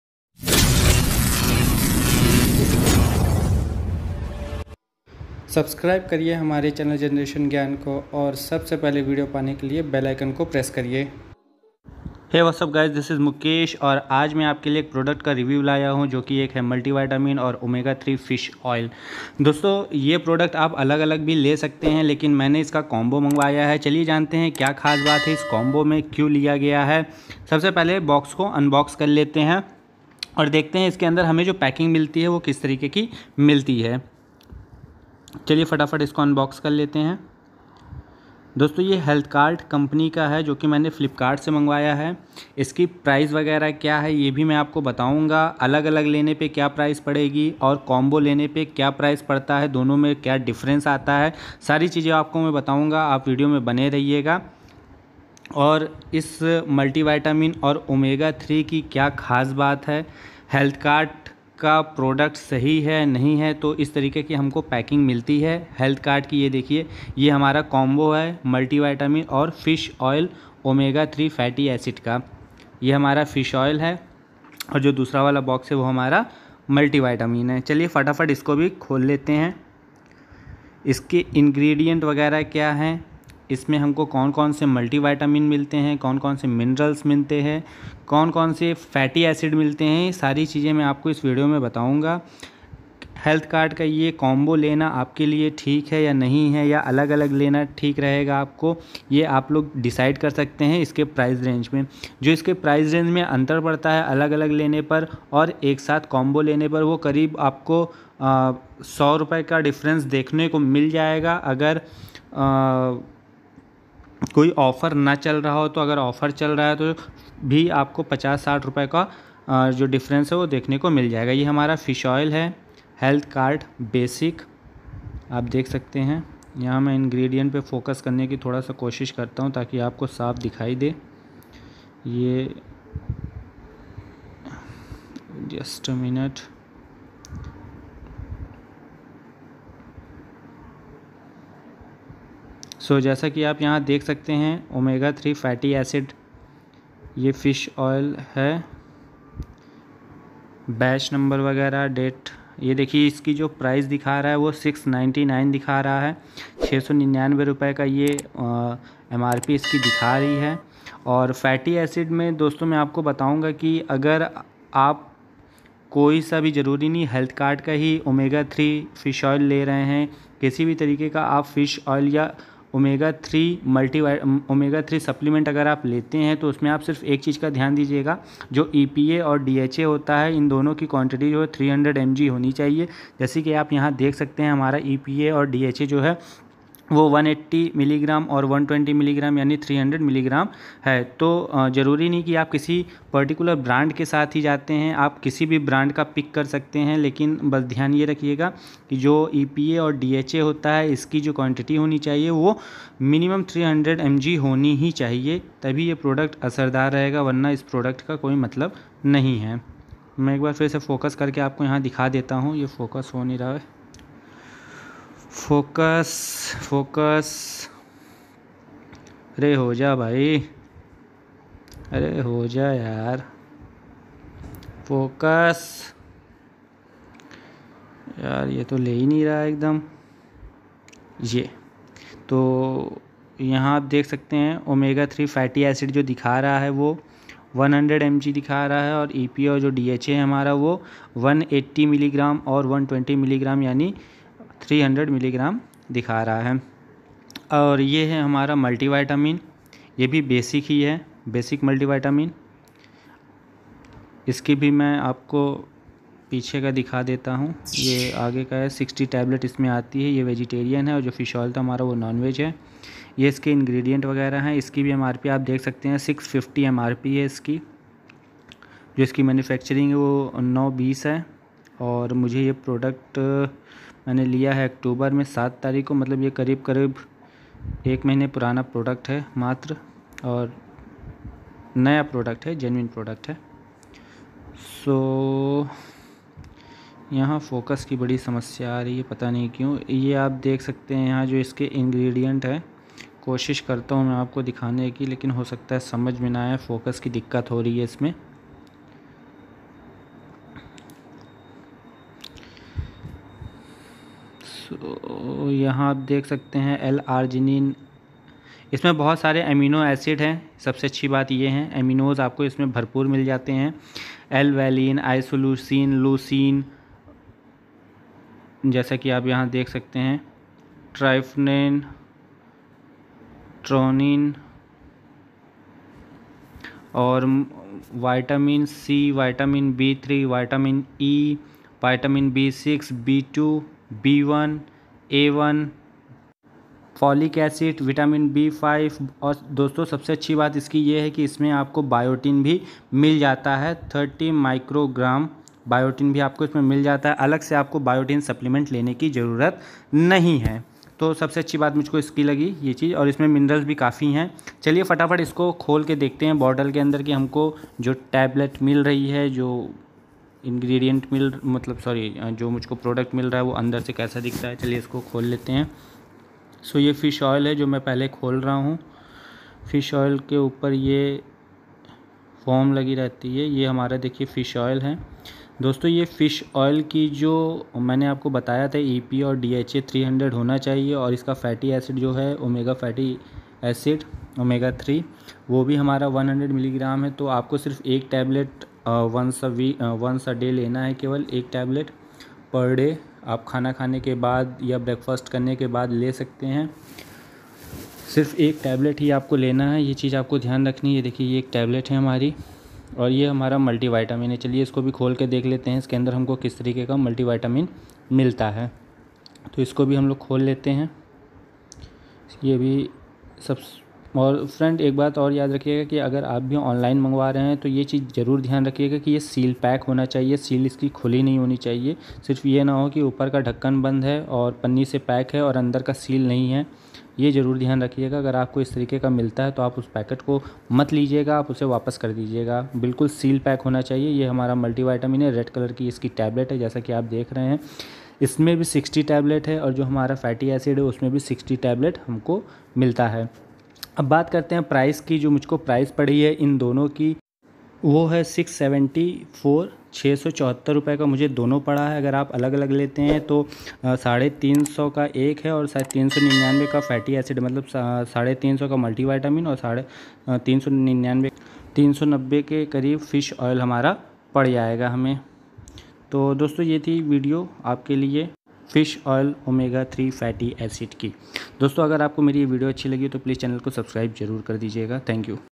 HealthKart multivitamin and Omega 3 unboxing and full review in Hindi cheap price combo लेना सही है?
hello friends aaj Ham review karne ja rahe hain
HealthKart multivitamin Omega 3 fatty acid unboxing and full review in Hindi kya HealthKart multivitamin or Omega 3 fatty acid Lena sahi hai Iske bare mein aapko is video mein sari Jankari mil Jayegi

Apple Mac book 8GB Ram 256 GB SSD at cheap price